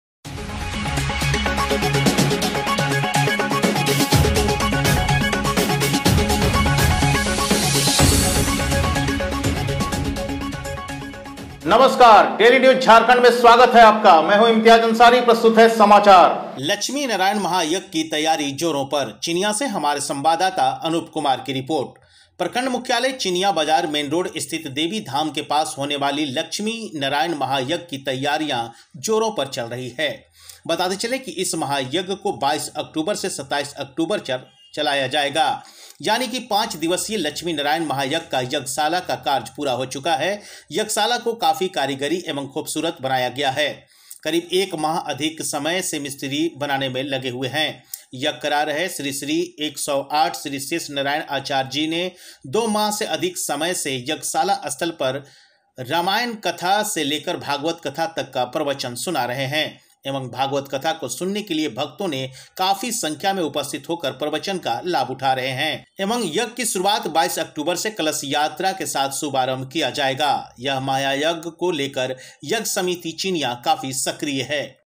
लक्ष्मीनारायण महायज्ञ की तैयारी जोरों पर।।DAILY_NEWS_JHARKHAND
लक्ष्मीनारायण महायज्ञ की तैयारी जोरों पर।

प्रखंड चिनियां से संवादाता अनूप कुमार का रिपोर्ट।

प्रखंड मुख्यालय चिनियां बाजार मेन रोड स्थित देवी धाम के पास होने वाली लक्ष्मी नारायण महायज्ञ की तैयारीया जोरों पर चल रही है। बताते चलें की इस महायज्ञ को 22 अक्टूबर से 27 अक्टूबर तक चलाया जायेगा। यानी की 5 दिवसीय  लक्ष्मी नारायण महायज्ञ का यज्ञशाला का कार्य पूरा हो चुका है। यज्ञशाला को काफी करीगरी एवं खूबसूरत बनाया गया है। करीब एक माह अधिक समय से मिस्त्री बनाने में लगे है। यज्ञ करा रहे श्री श्री 108 श्री शेष नारायण आचार्य जी ने 2 माह से अधिक समय से यज्ञशाला स्थल पर रामायण कथा से लेकर भगवत कथा तक का प्रवचन सुना रहे है। एवं भगवत कथा को सुनने के लिए भक्तो ने काफी संख्या में उपस्थित होकर प्रवचन का लाभ उठा रहे है। एवं यज्ञ की शुरुवात 22 अक्टूबर से कलश यात्रा के साथ सुभारंभ किया जाएगा। इस महायज्ञ को लेकर यज्ञ समिति चिनियां काफी सक्रिय है।